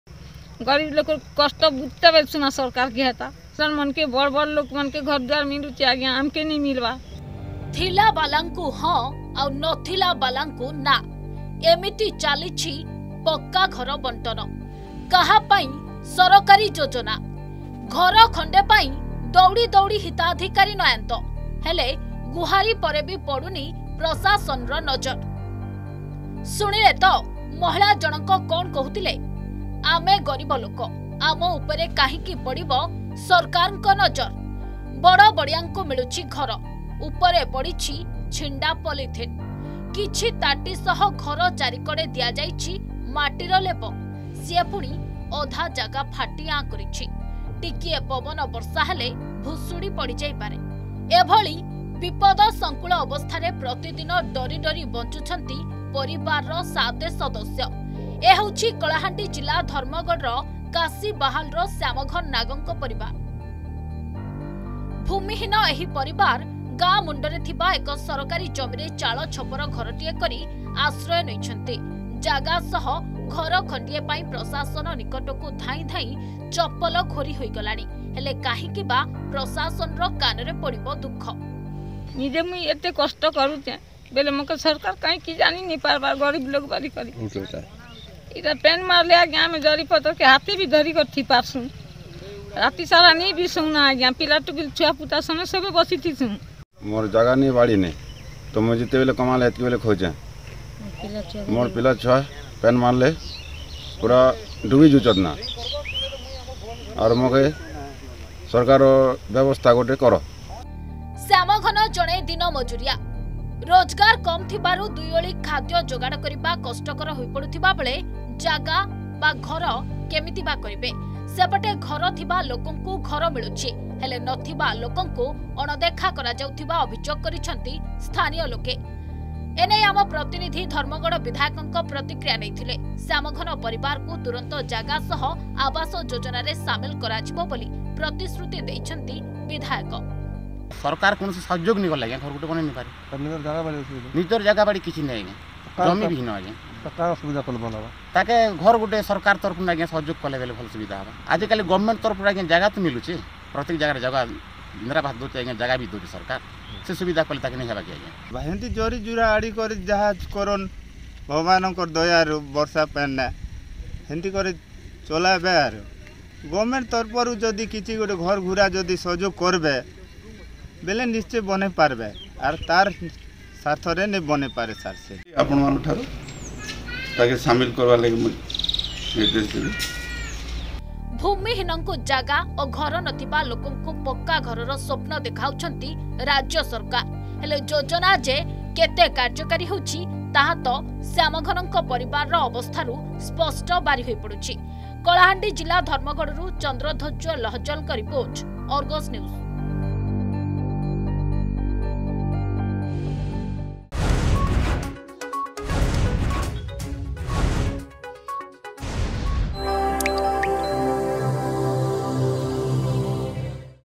ଥିଲା ବାଲାକୁ ହଁ ଆଉ ନଥିଲା ବାଲାକୁ ନାଁ: Beneficiaries Struggle To Get Pucca Houses Under Awas Yojana
ଏମିତି ଚାଲିଛି ପକ୍କାଘର ବଣ୍ଟନ. ଥିଲା ବାଲାକୁ ହଁ ଆଉ ନଥିଲା ବାଲାକୁ ନାଁ I ଘର ଖଣ୍ଡେ ପାଇବା ପାଇଁ ଦୌଡିଦୌଡି ହିତାଧିକାରୀ ନୟାନ୍ତ ହେଲେ. ଗୁହାରି ପରେ ବି ପଡୁନି ପ୍ରଶାସନର ନଜର